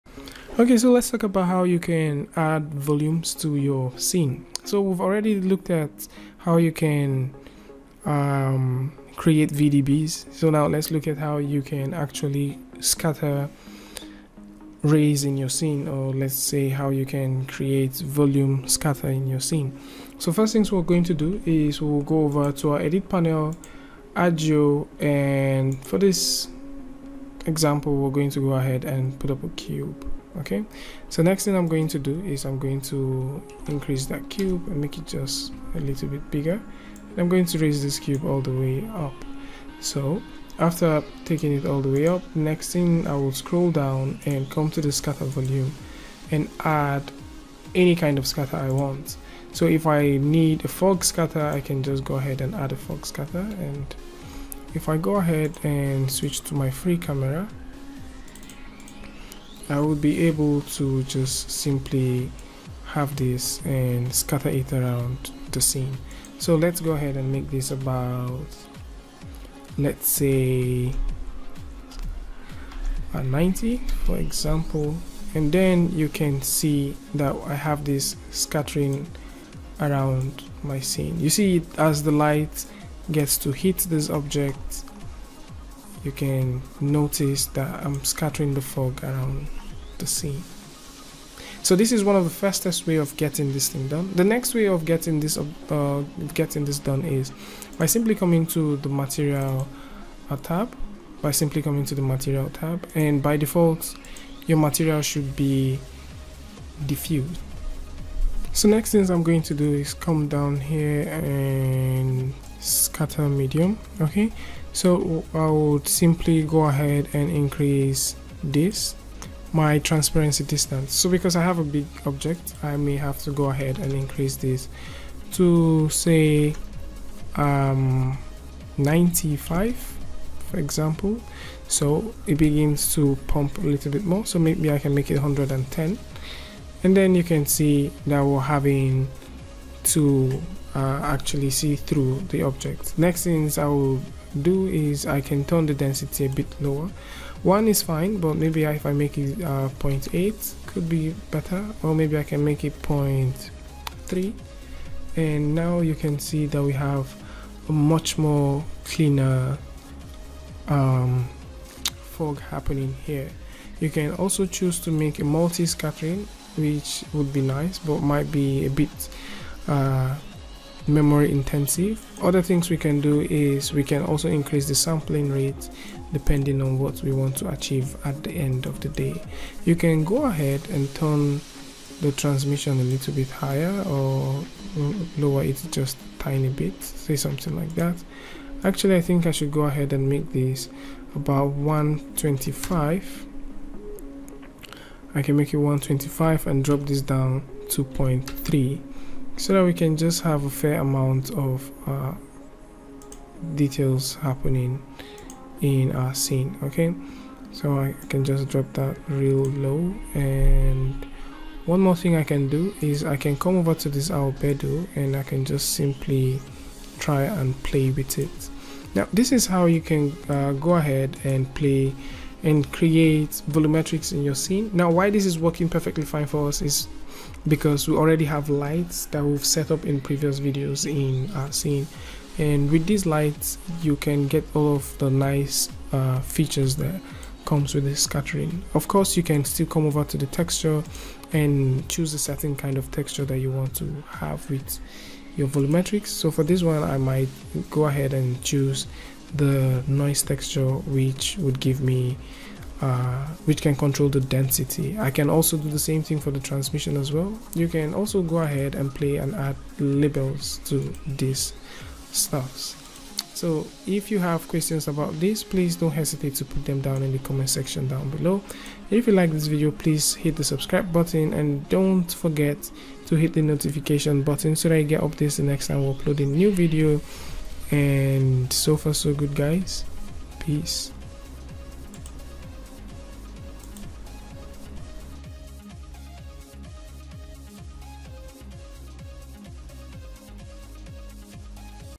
KEYSHOT 8 - FOG AND VOLUMETRIC LIGHTING
Let's have a look at the new volumetric and fog feature that exists in keyshot8